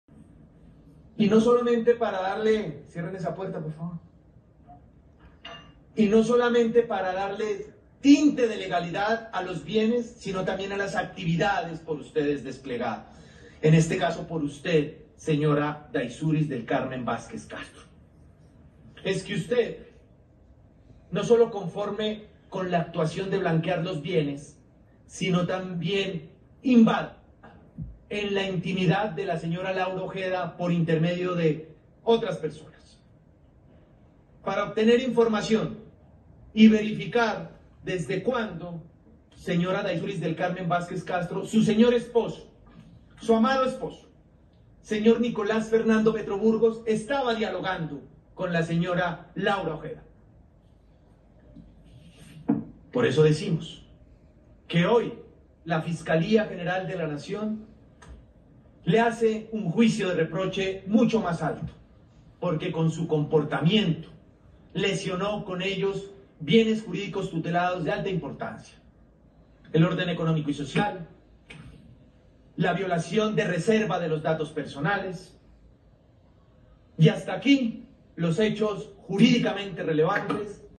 Audiencia de imputación de cargos contra Nicolás Petro y Day Vásquez / parte 2
El juez 74 Penal Municipal con Función de Control de Garantías antes hizo varias advertencias y dejó varias constancias de las audiencias que fueron de carácter reservado.

Que legalizaba la expedición de dos ordenes de allanamientos y registro por cumplirse las previsiones constitucionales y legales y se declaró legal el registro de allanamiento de dos inmuebles en donde se capturaron a Nicolás Petro y Daysuris Vásquez, así como la incautación con fines de comiso, $24.9 millones y la legalización con fines probatorio.